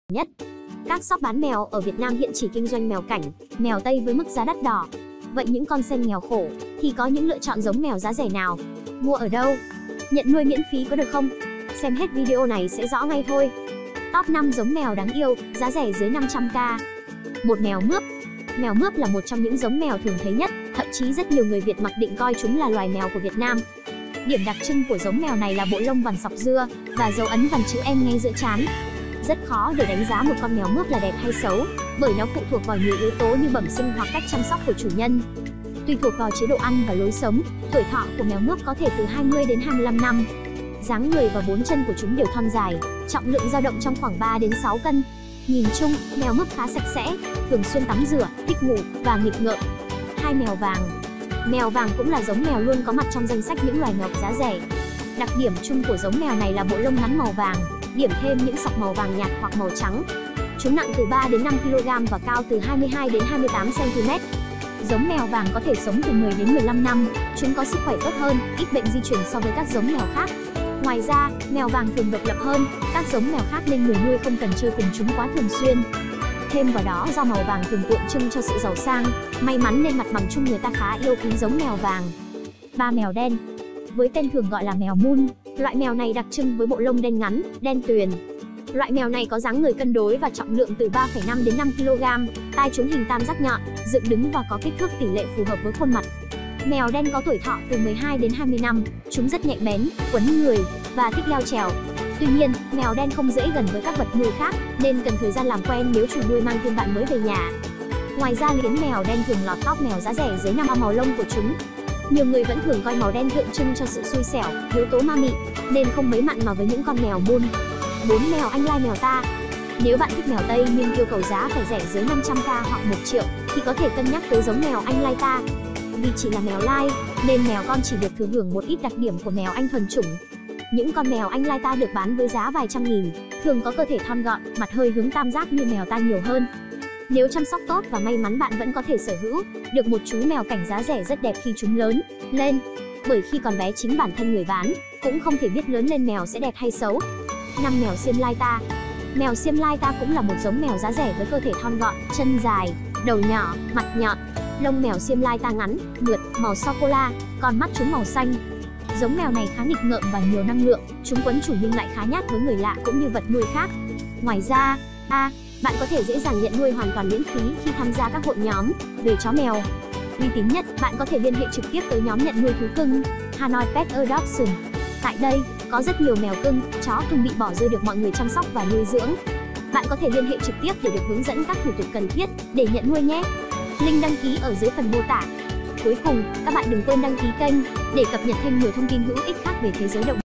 Cách mua mèo cảnh đẹp giá siêu rẻ dưới 50OK và địa chỉ xin nuôi miễn phí
Mua mèo cảnh dưới 500k liệu có được không? Với 500k thì mua được những giống mèo nào. Nếu sen không có tiền thì phải làm cách nào để có thể nuôi mèo đây?